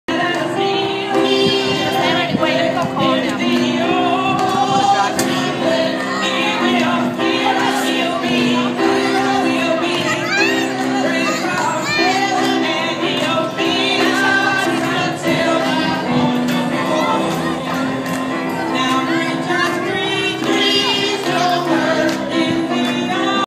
Beneath The Balconies 2012
Gospel Choirs Sing on Main Street New Iberia.